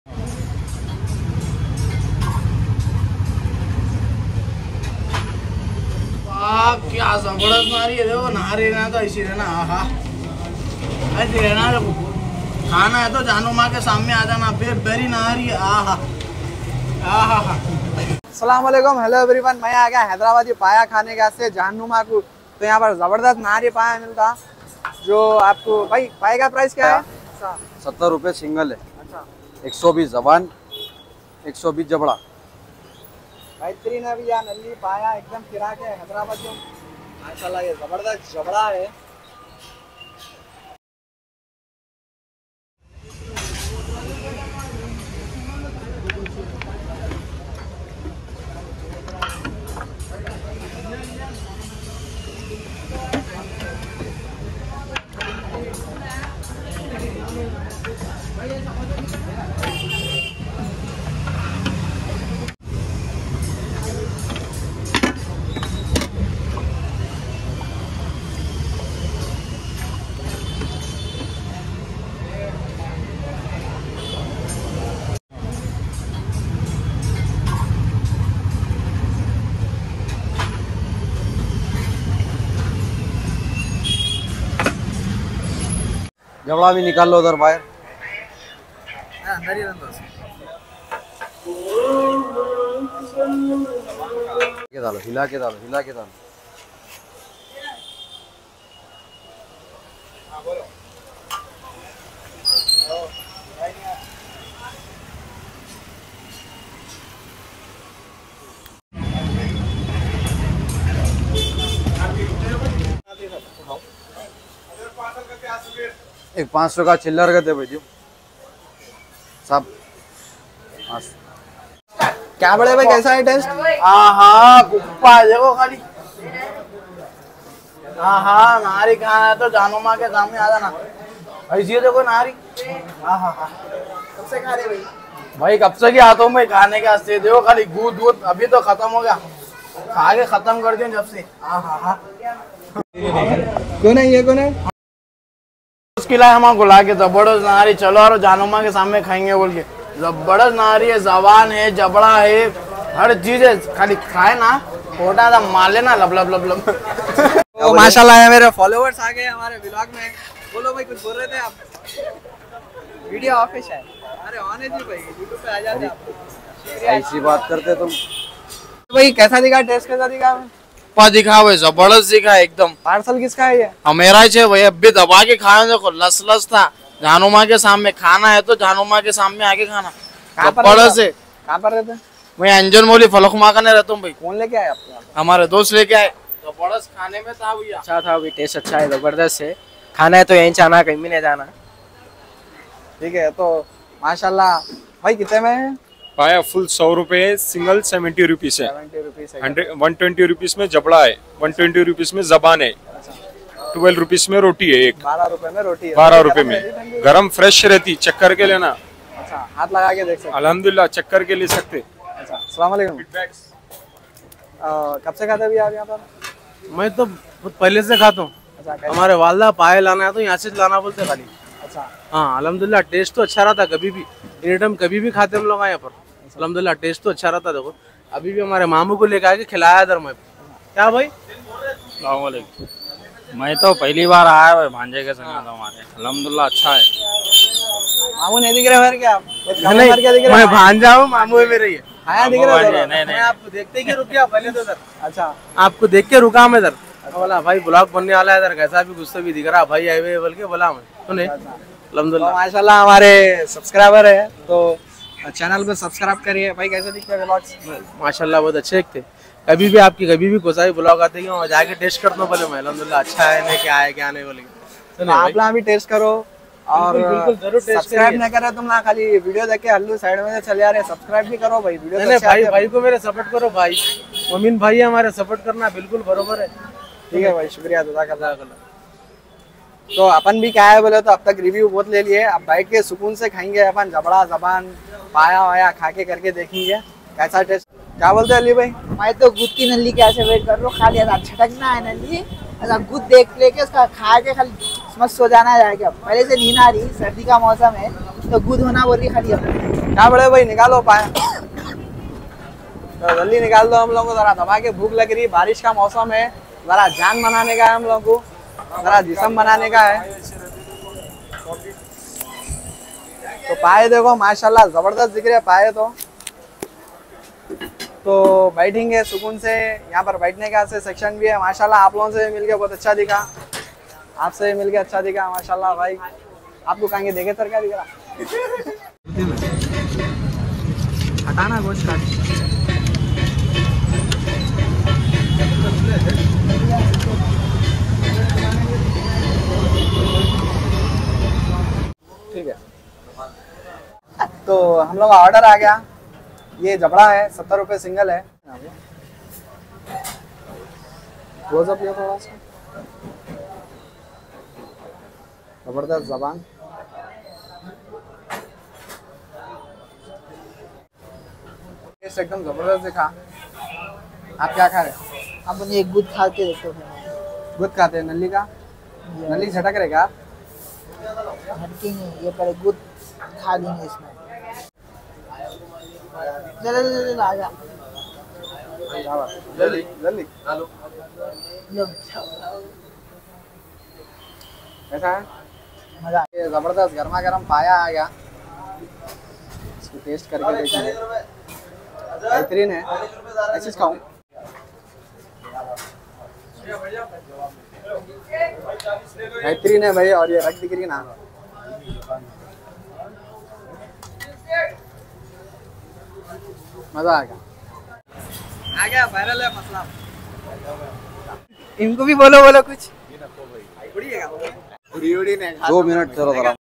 Authentic hyderabadi nihari paya | Indian street food
Title: "Exploring Jahanuma's Best Street Food: Mouth-Watering Nihari & Paya!"**

**Description:**

🤤 **Get ready to drool!** Join us on a flavor-packed journey through the bustling streets of Jahanuma, Hyderabad, where we uncover the secrets behind the legendary **Nihari** and **Paya**. These traditional delicacies are cooked to perfection, simmering for hours to create a burst of rich, aromatic flavors that you won't find anywhere else.

- Discover the vibrant street food scene in Jahanuma.
- Watch the master chefs as they craft the perfect Nihari and Paya.
- Experience the mouth-watering taste and unique textures that make these dishes local favorites.
- Hear from locals and food enthusiasts about why these dishes are a must-try!